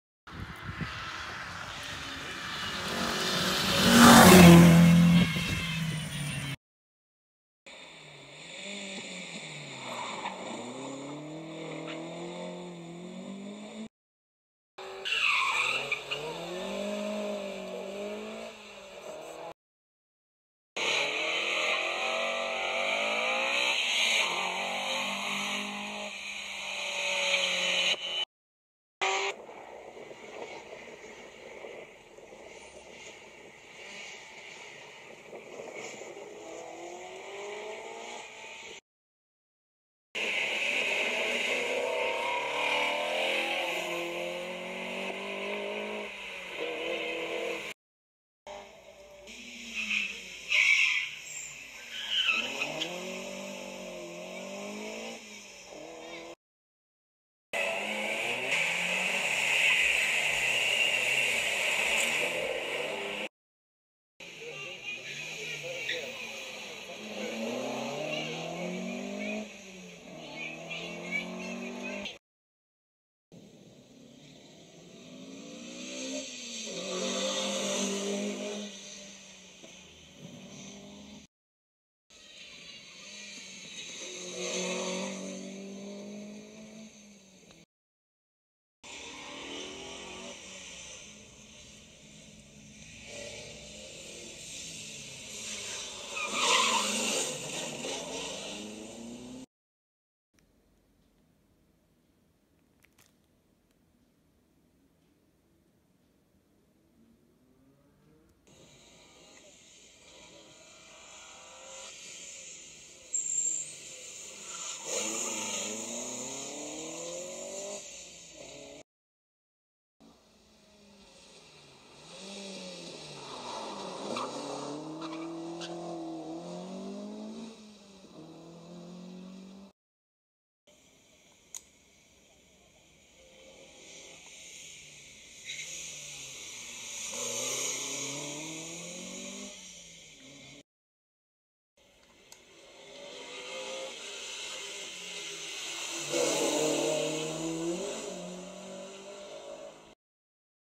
BMW 318ti compact m42 rally car
Competing in the jersey rally 2018